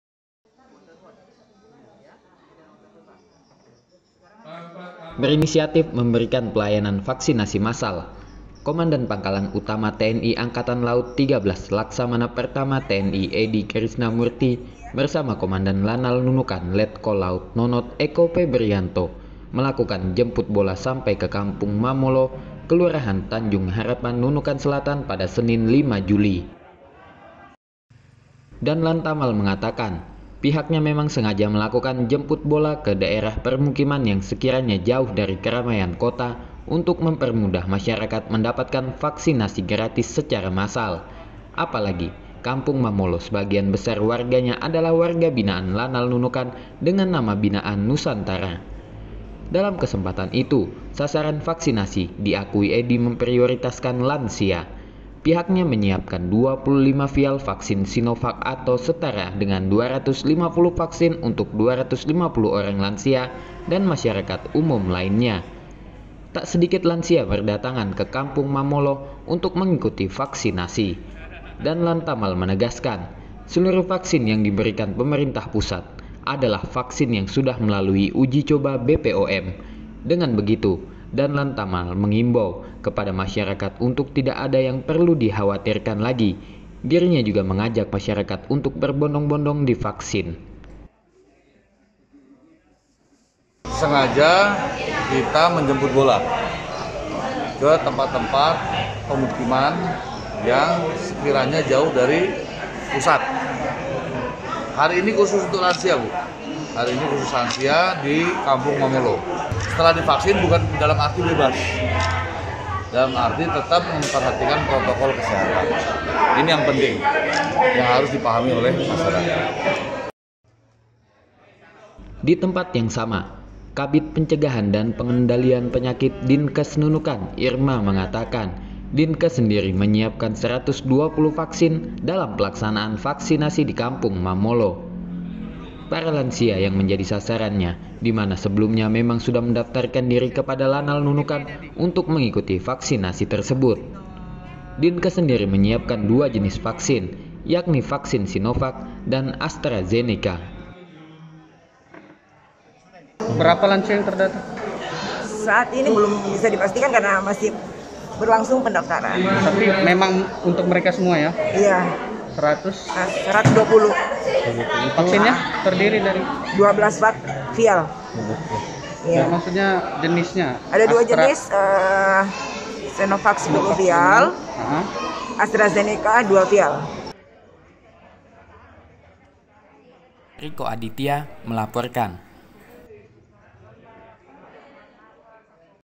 Danlantamal XIII Jemput Bola Vaksinasi ke Permukiman Jauh dari Kota
Berinisiatif memberikan pelayanan vaksinasi massal, Komandan Pangkalan Utama TNI AL XIII, Laksamana Pertama TNI, Edi Krisna Murti bersama Komandan Lanal Nunukan, Letkol Laut (P) Nonot Eko Febrianto,  jemput bola sampai ke Kampung Mamolo, Kelurahan Tanjung Harapan, Nunukan Selatan pada Senin 5 Juli.

Reporter: Riko Aditya